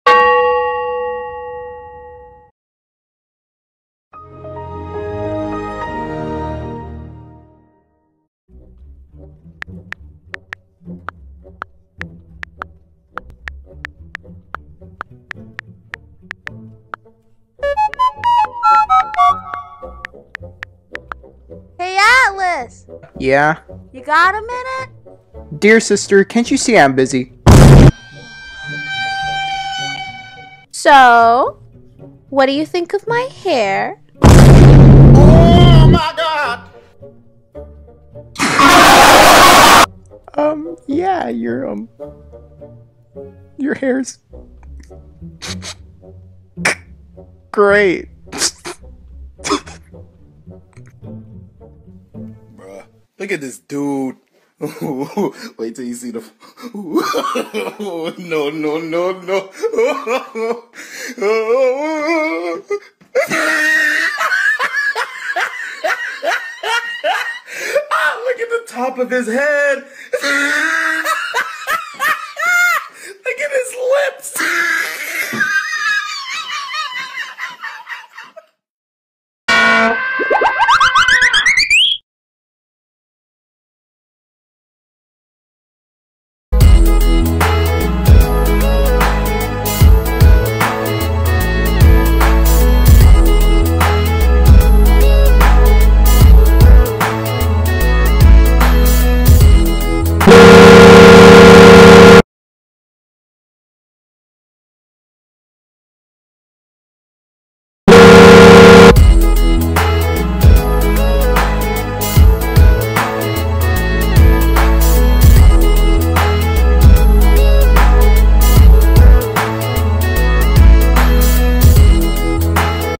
“Visual Changes” | SVC | Prologue 2
As Atlas Is Minding His Own Business, His Sister, Astra, Atlas Sister, Wanted To Know What Atlas Thinks About The New Change She Did To Her Hair

Animation Done By Me!

Atlas Voice - Me
Eleanors Voice - My Sister

All Credit For Music & Sounds Belongs To Their Respectful Creators!

Look At The Top Of His Head Meme: Belongs To Original Creator Of Meme, Found The Audio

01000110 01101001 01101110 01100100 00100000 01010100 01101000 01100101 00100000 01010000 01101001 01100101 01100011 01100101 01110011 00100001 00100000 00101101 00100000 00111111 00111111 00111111